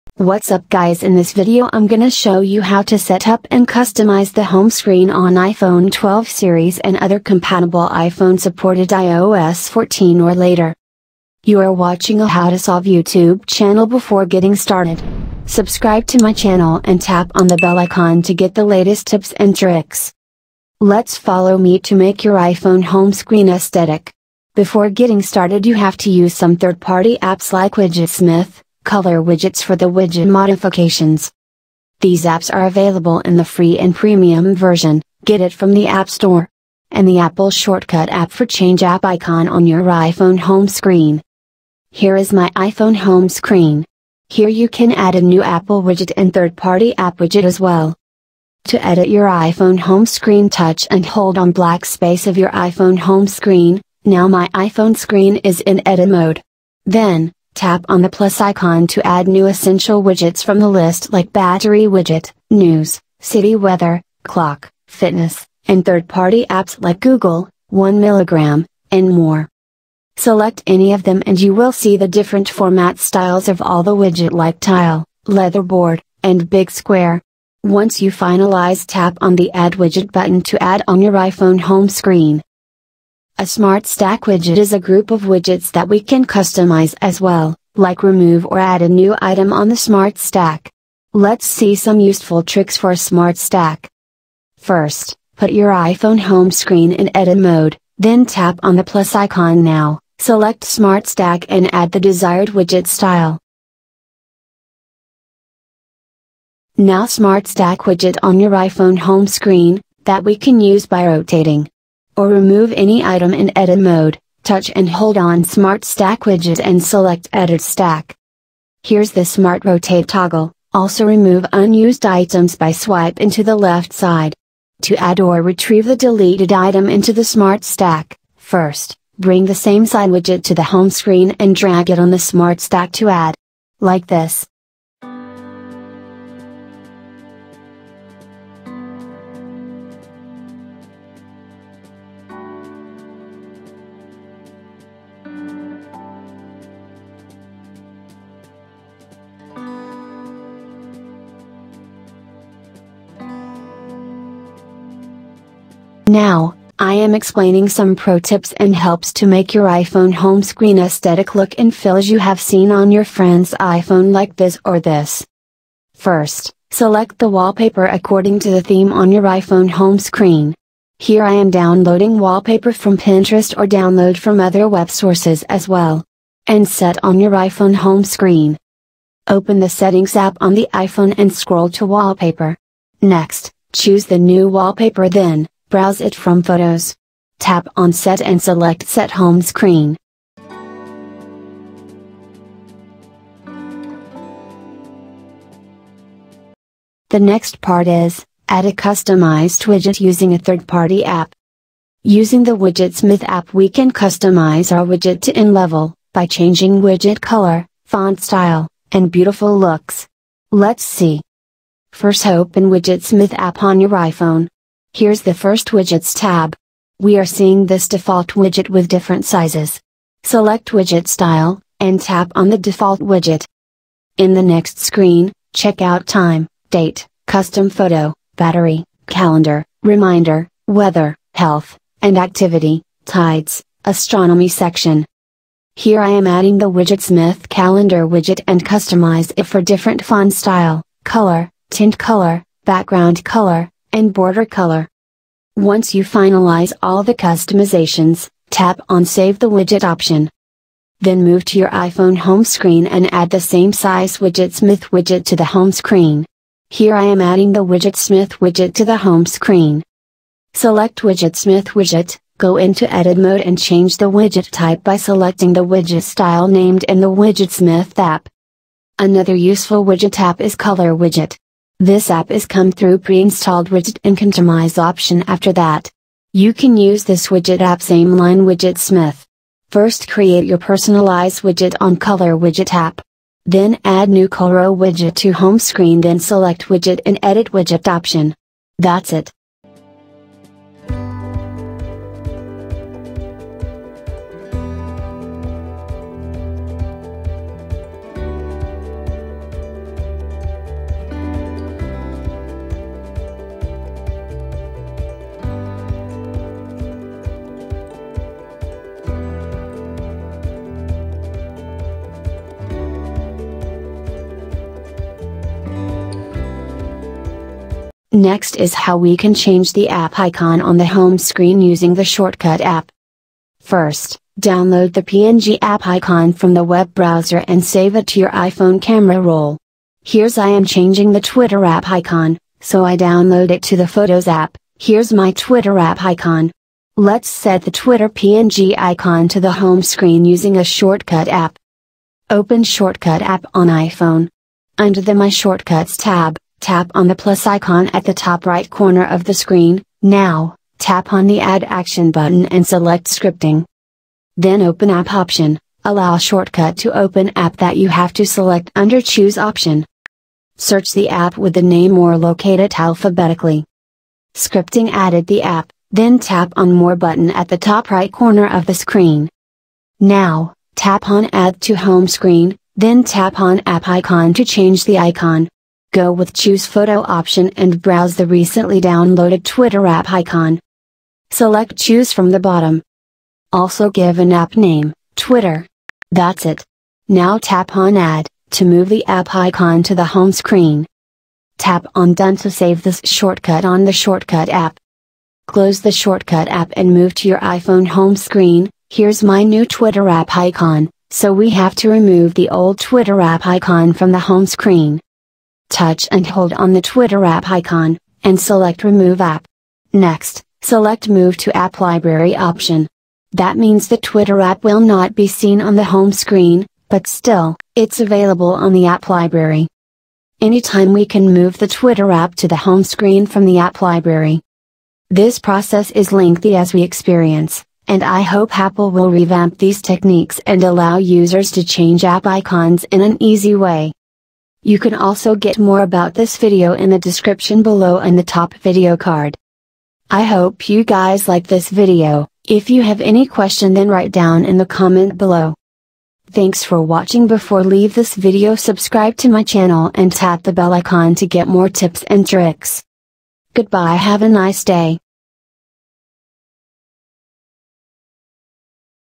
iPhone 13 Pro Max/ 12 Mini: How to Customize & Setup Home screen ideas | Aesthetic Widget & App icon
ios 15 home screen ideas Customization and Setup Home screen on iPhone 13 pro max, iPhone 12, iPhone 12 Mini, iPhone 12 Pro, iPhone 12 Pro Max running on iOS 14. Modify Widget and App icon on your iPhone home screen. Change home page Aesthetic look in free Using some free apps and Shortcuts app on your iPhone. Change App icon on the iPhone 12 Home screen.

Timestamp:
00:00 - intro
00:28 - Used apps
00:49 - Add new Apple & Third-Party Widget
01:24 - Use and Customize Smart Stack Widget
02:37 - Aesthetic Home screen look and feel
03:21 - Use Widgetsmith widget app
04:34 - Use Color Widget App
05:19 - Change app icon
06:42 - Remove Duplicate app icon from the home screen
07:19 - Outro

Used Apps:
1. Widgetsmith,
2. Color Widgets for the widget modifications.
3. Shortcuts App

These apps are available in the Free and premium version,

Customize Home screen & Smart Stack Widget

To edit your iPhone home screen touch and hold on black space of your iPhone home screen, Now my iPhone screen is in edit mode. then, Tap on the Plus icon to add new essential widgets from the list like Battery widget, News, City Weather, Clock, Fitness, and Third-party apps like Google, 1mg, and more.

Select any of them and You will see the different format styles of all the widget like tile, Leatherboard, and Big Square. Once you finalize tap on the "Add Widget button" to add on your iPhone home screen.

A smart stack widget is a group of Widgets that we can customize as well, like Remove or add a new item on the Smart stack. Let's see some useful tricks for a smart stack.

First, put your iPhone home screen in edit mode, then Tap on the plus icon now, Select Smart Stack and Add the desired widget style.

Now Smart stack widget on your iPhone home screen, That we can Use by rotating. or Remove any item in edit mode, Touch and hold on Smart Stack widget and Select Edit Stack.

Here's the smart rotate toggle, also Remove unused items by swipe into the left side. To add or Retrieve the deleted item into the smart stack, First, bring the same side widget to the home screen and Drag it on the Smart stack to add. like this.

Now, I am Explaining some pro tips and helps to make your iPhone home screen aesthetic look and feel as you have seen on your Friends iPhone like this or this.

First, select the Wallpaper according to the theme on your iPhone home screen. here I am downloading wallpaper from Pinterest or Download from other web sources as well. and Set on your iPhone home screen.

Open the Settings app on the iPhone and scroll to Wallpaper. Next, Choose the new Wallpaper then, Browse it from Photos. Tap on set and Select Set home screen.

Using Third-Party Widget App

Using the Widgetsmith app we can customize our Widget to End level, By changing widget color,  Font style, and Beautiful looks. Let's see.

First Open Widgetsmith app on your iPhone. Here's the First Widgets tab. We are seeing this Default Widget with different sizes. Select widget style, and Tap on the Default widget.

in the next screen, checkout Time, Date, Custom Photo, Battery, Calendar, Reminder, Weather, Health & Activity, Tides, Astronomy section.

Here I am adding the widgetsmith Calendar widget and Customize it for Different font style, color, Tint Color, Background Color, and Border Color.

Once you finalize all the customizations, Tap on Save the Widget option.

Then move to your iPhone home screen and Add the Same Size Widgetsmith widget to the Home screen. here I am adding the Widget smith widget to the home screen

Select Widget smith widget, Go into Edit mode and change the widget type by Selecting the widget style named in the Widgetsmith app.

Another Useful Widget app is a color widget. This app has come through a preinstalled widget and Contomize option after that. You can use This Widget app same line Widget smith. first create your personalize widget on Color widget app. then Add New Coloro widget to Home screen then Select widget in Edit widget option. that's it.

Next is how we can change the app icon on the Home screen using the Shortcut app.

First, download the png app icon.

Open shortcut app on iPhone. Under the My Shortcuts Tab, Tap on the Plus icon at the top right corner of the screen, Now, Tap on the Add Action button and Select Scripting.

Then Open App option, Allow Shortcut to Open App that you have to select under choose option.

Search the app with the name or locate it alphabetically.

Scripting added the app, then Tap on more button at the top right corner of the screen.

Now, Tap on Add to Home screen, Then Tap on App icon to change the icon. Go with the Choose Photo option and Browse the recently downloaded Twitter app icon.

Select Choose from the bottom.

Also Given App name, Twitter. that's it. Now Tap on Add, To move the App icon to the home screen.

Tap on Done to save this Shortcut on the Shortcut app.